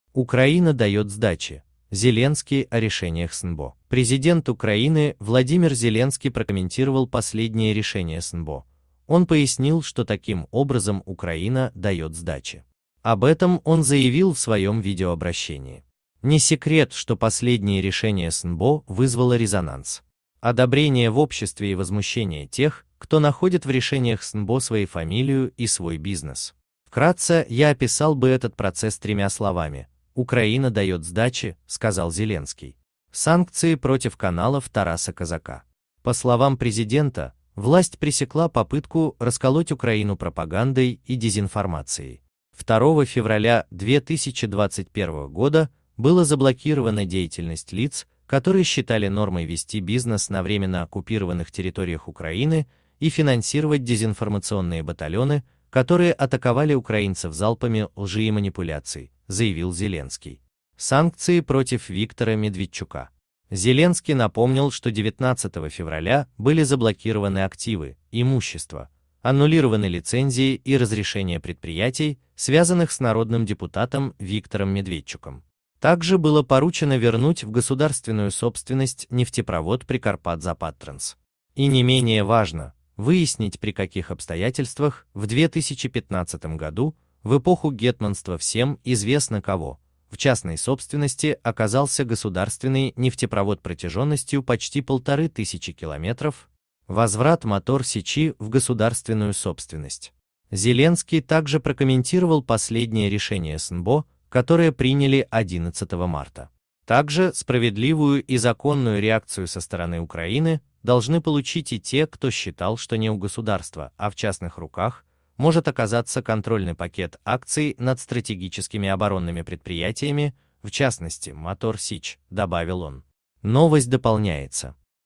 Украина дает сдачи: Зеленский о решениях СНБО.
Новости Политика СНБО Президент Украины Владимир Зеленский прокомментировал последние решения СНБО. Он пояснил, что таким образом Украина "дает сдачи". Об этом он заявил в своем видеообращении. "Не секрет, что последние решение СНБО вызвало резонанс. Одобрение в обществе и возмущение тех, кто находит в решениях СНБО свои фамилию и свой бизнес. Вкратце, я описал бы этот процесс тремя словами - "Украина дает сдачи", - сказал Зеленский. Санкции против каналов Тараса Козака.
По словам президента, власть пресекла попытку ра